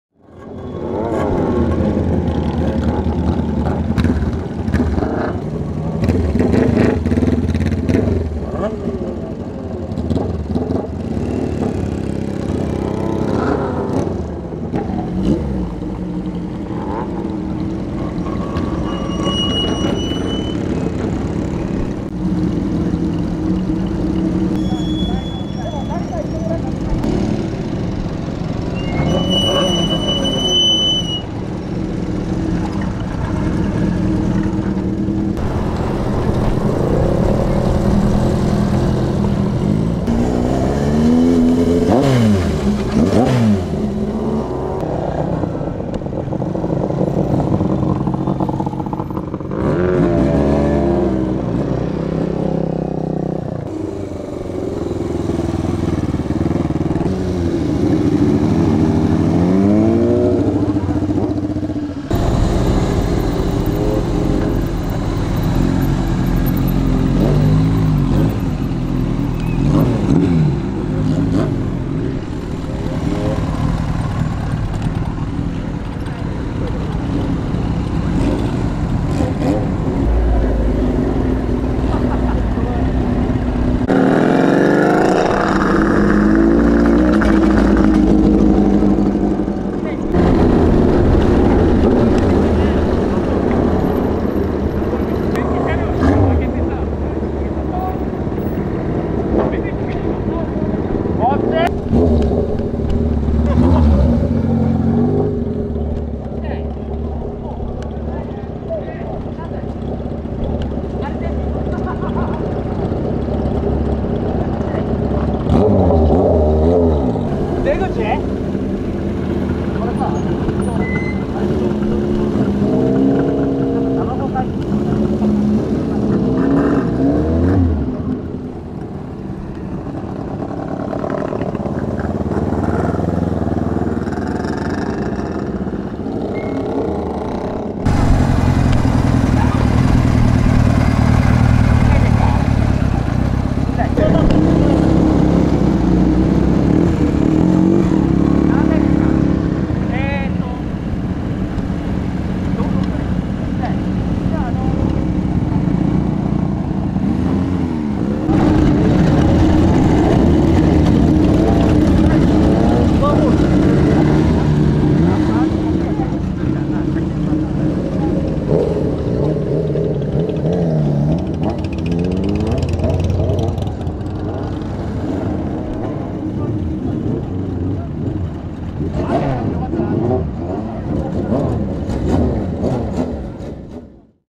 第二駐車場も満車❗️ 2021富士河口湖 オートジャンボリー❽
2021.6/20    7th

オートジャンボリー2021

カッコイイ参加車両を撮影❗️

旧車四輪　　旧車二輪
オートジャンボリー内映像